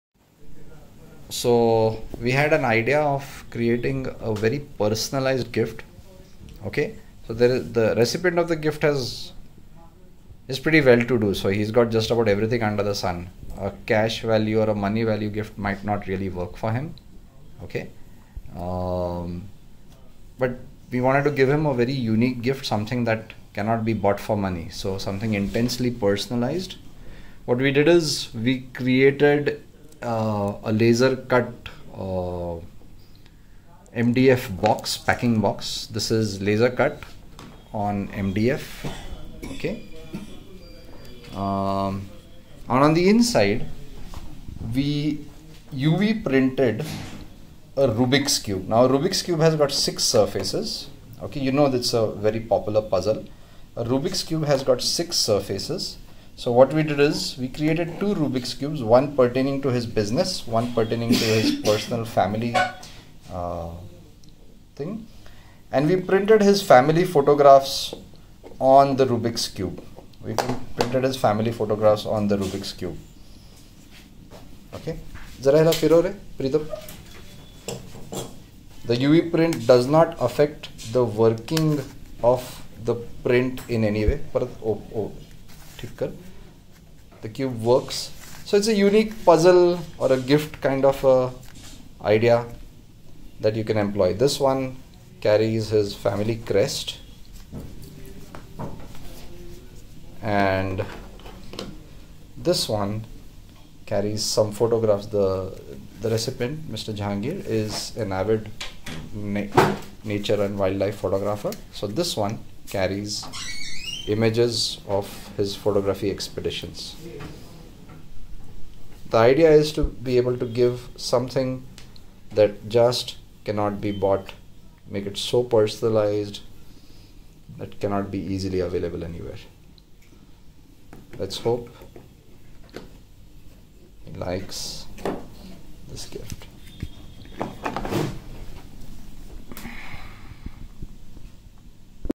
Printed Rubik's Cube Bearing Personalized Photos - A Fabulous Gifting Idea
We printed photos on a Rubik's Cube to create a  highly personalised gift.

The printing was done using a UV flatbed printer.